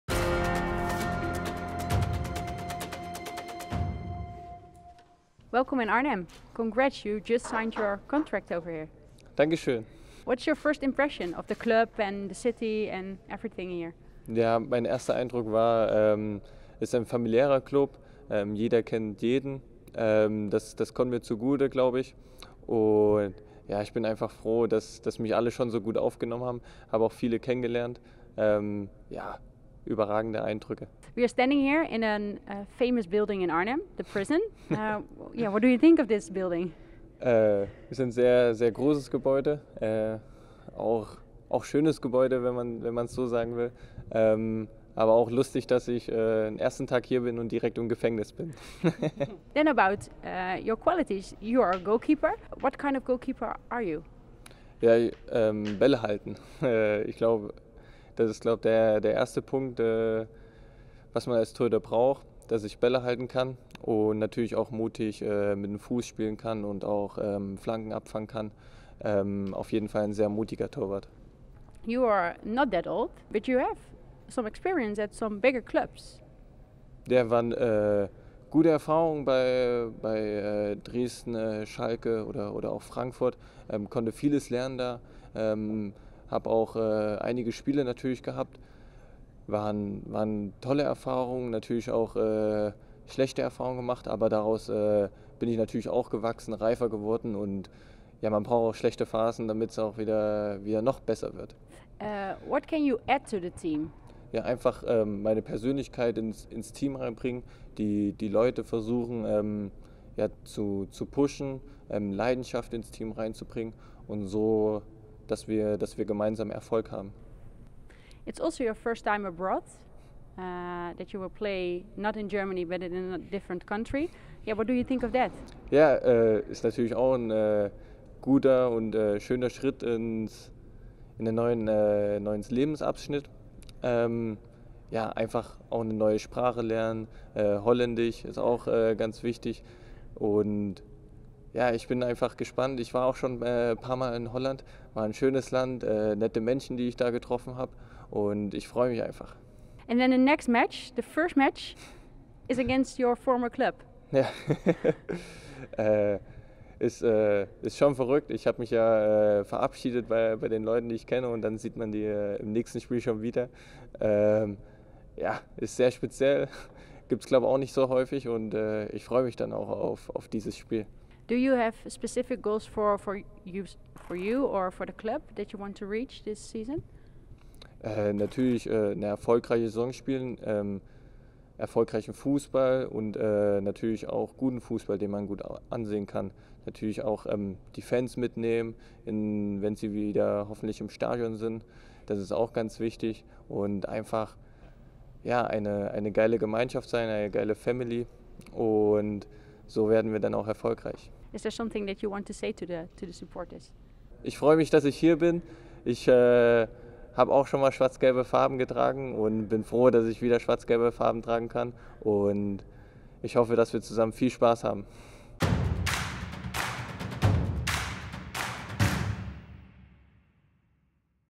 Markus Schubert vult vacante keeperspositie in bij Vitesse
Markus Schubert mag zich officieel de nieuwe doelman van Vitesse noemen. De 23-jarige sluitpost heeft zijn handtekening gezet onder een contract tot medio 2024 in Arnhem, met een optie voor nog een seizoen. De opvolger van Remko Pasveer is afkomstig van Schalke 04.